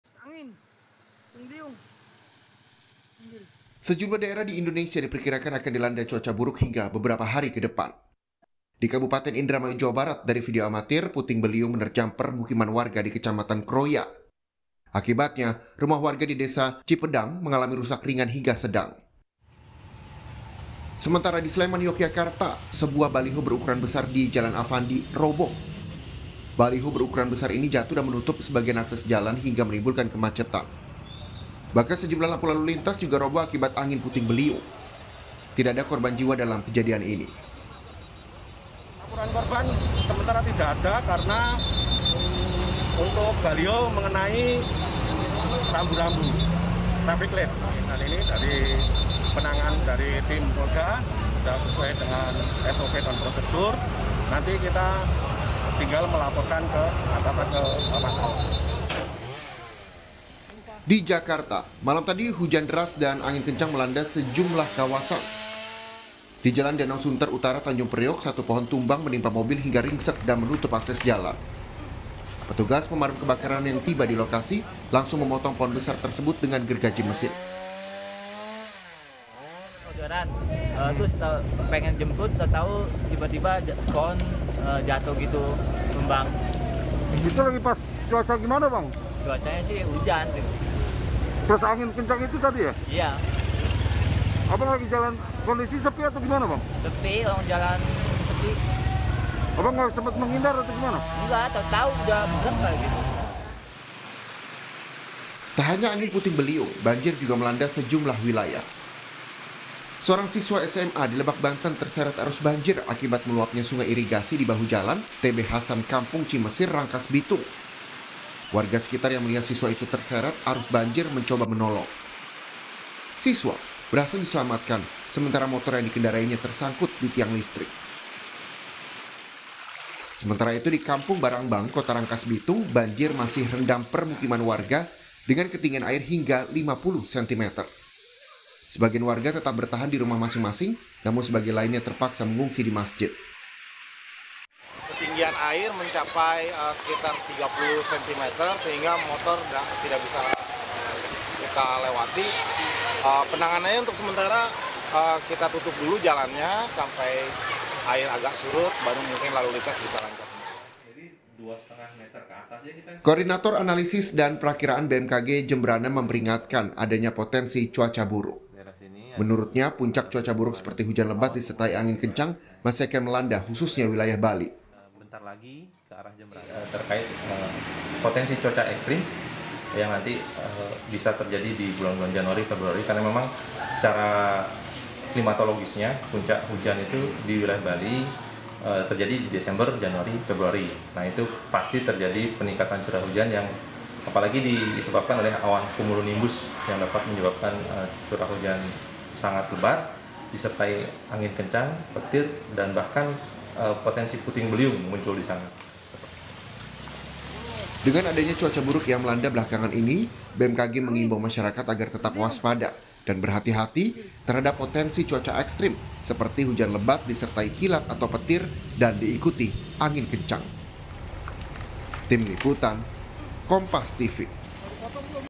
Indonesia Masih Berpotensi Cuaca Buruk, BMKG Imbau Warga Waspadai Hujan Deras dan Angin Kencang
JAKARTA, KOMPAS.TV - Sejumlah daerah di Indonesia diperkirakan akan dilanda cuaca buruk hingga beberapa hari ke depan.

Di Kabupaten Indramayu, Jawa Barat, dari video amatir, puting beliung menerjang permukiman warga di Kecamatan Kroya.

Akibatnya, rumah warga di Desa Cipedang,mengalami rusak ringan hingga sedang.

Sementara di Sleman Yogyakarta, sebuah baliho berukuran besar di Jalan Afandi roboh.

Baliho berukuran besar ini jatuh dan menutup sebagian akses jalan hingga menimbulkan kemacetan.

Bahkan sejumlah lampu lalu lintas juga roboh akibat angin putting beliung.

Tidak ada korban jiwa dalam kejadian ini.

Di Jakarta, malam tadi ujan deras dan angin kencang melanda sejumlah kawasan.

Di Jalan Danau Sunter Utara, Tanjung Priok, satu pohon tumbang menimpa mobil hingga ringsek dan menutup akses jalan.

Petugas pemadam kebakaran yang tiba di lokasi langsung memotong pohon besar tersebut dengan gergaji mesin.

Tak hanya angin puting beliung, banjir juga melanda sejumlah wilayah.

Seorang siswa SMA di Lebak, Banten terseret arus banjir akibat meluapnya sungai irigasi di bahu Jalan Tb Hasan Kampung Cimesir, Rangkasbitung.

Siswa berhasil diselamatkan, sementara motor yang dikendarainya tersangkut di tiang listrik.

Sementara itu, di Kampung Barangbang, Kota Rangkasbitung banjir masih rendam permukiman warga dengan ketinggian air hingga 50 sentimeter.

Koordinator Analisis dan Prakiraan BMKG Jembrana memperingatkan adanya potensi cuaca buruk.

Menurutnya, puncak cuaca buruk seperti hujan lebat disertai angin kencang masih akan melanda khususnya wilayah Bali.

Dengan adanya cuaca buruk yang melanda belakangan ini, BMKG mengimbau masyarakat agar tetap waspada dan berhati-hati terhadap potensi cuaca ekstrem seperti hujan lebat disertai kilat atau petir dan diikuti angin kencang.